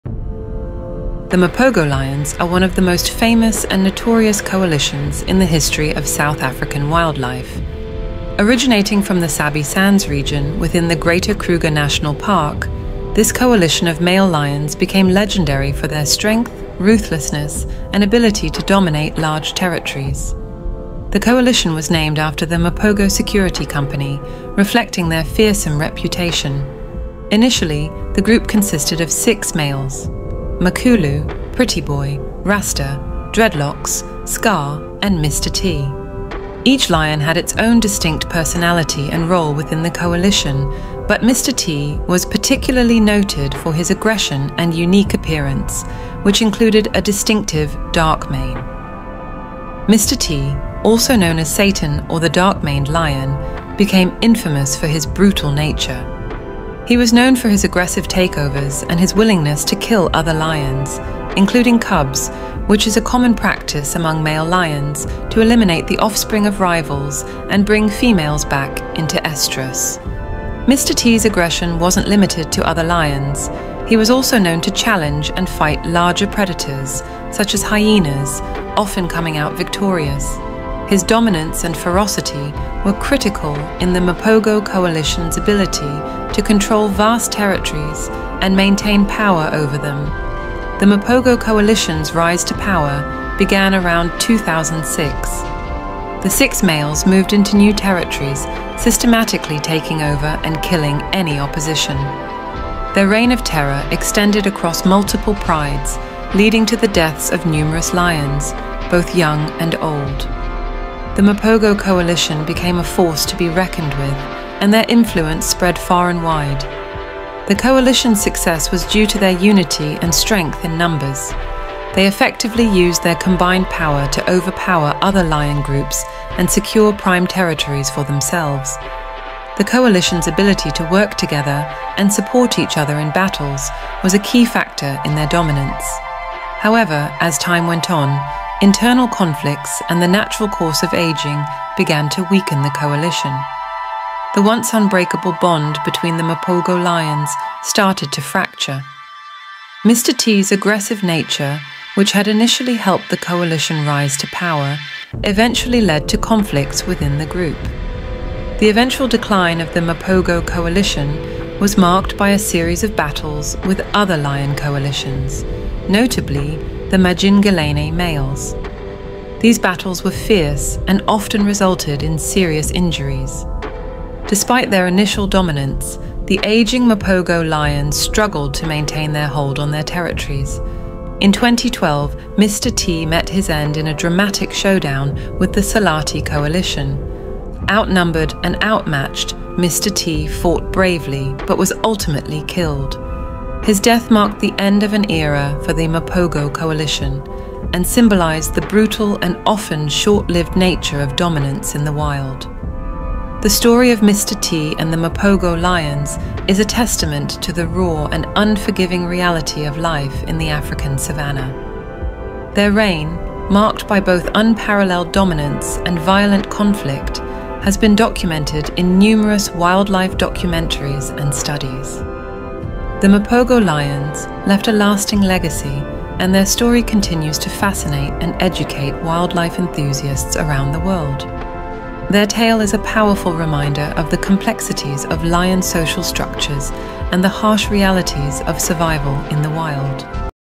The Legendary Tale of Mr. T
Dive into the gripping saga of Mr. T and the notorious Mapogo lion coalition, one of the most fearsome and legendary lion groups in South African wildlife history. Join us as we explore their rise to power, brutal dominance, and ultimate downfall.Experience the raw power and intense drama of nature's fiercest predators. This story is a testament to the brutal and often short-lived nature of dominance in the wild.